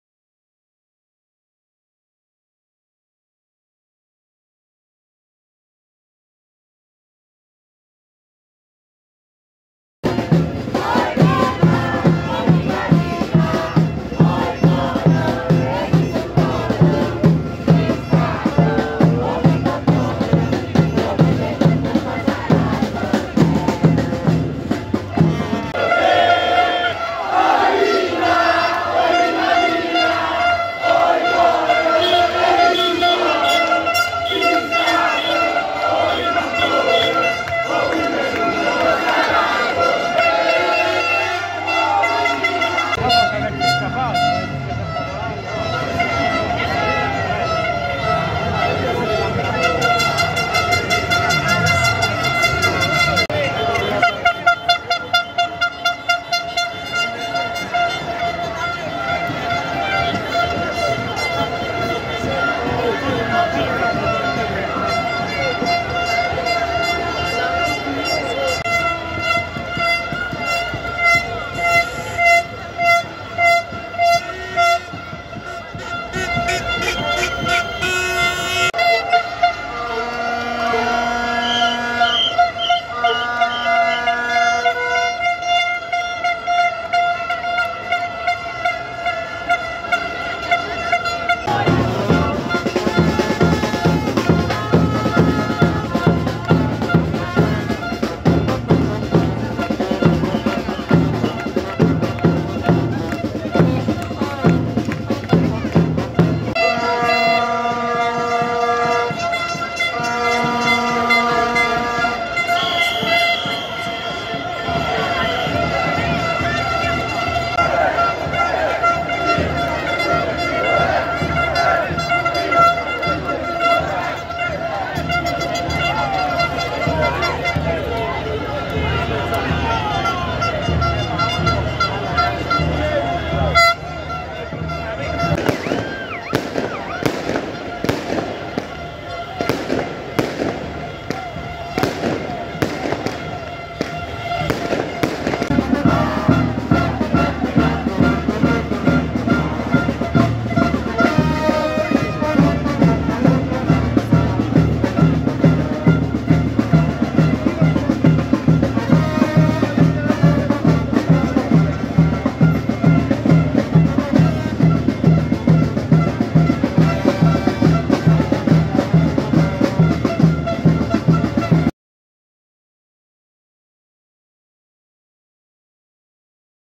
La festa a Pompei per lo Scudetto del Napoli
A Pompei i tifosi del Napoli sono scesi in piazza Bartolo Longo per festeggiare lo Scudetto vinto artimeticamente dopo il pareggio (1-1) di Udine. Al triplice fischio dell'arbitro la tensione dei tifosi azzurri si è sciolta in gioia e lacrime.